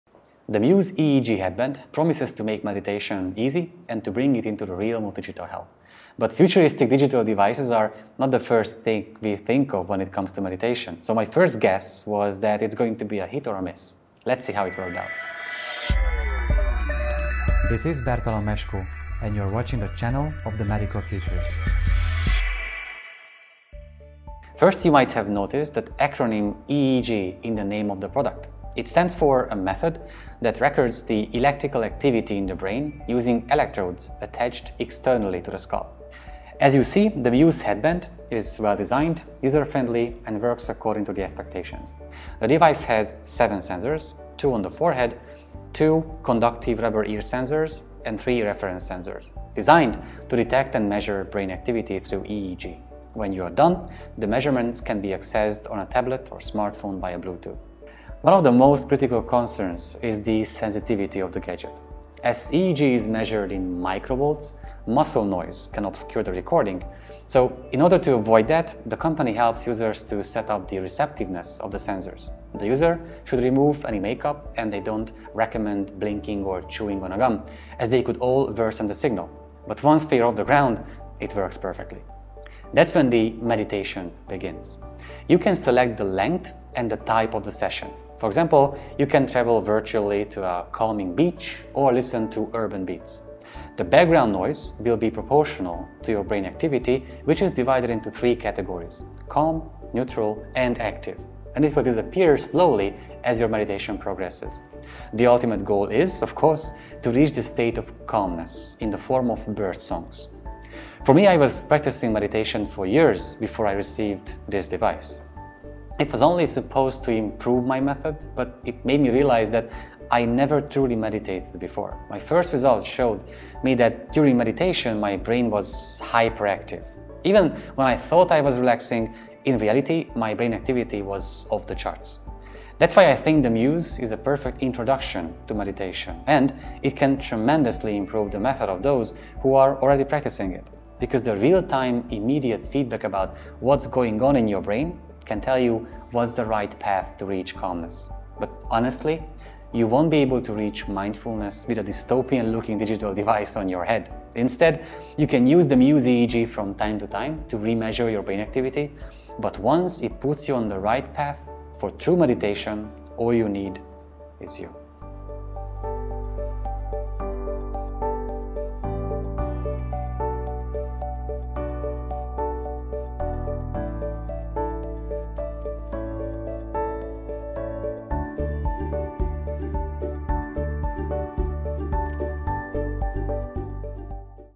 Muse EEG Headband Review - The Medical Futurist
The Muse EEG headband promises to make meditation easy and to bring it into the realm of digital health. But futuristic digital devices are not the first thing we think of when it comes to meditation, so my first guess was that it’s going to be a hit or a miss. Let’s see how it worked out.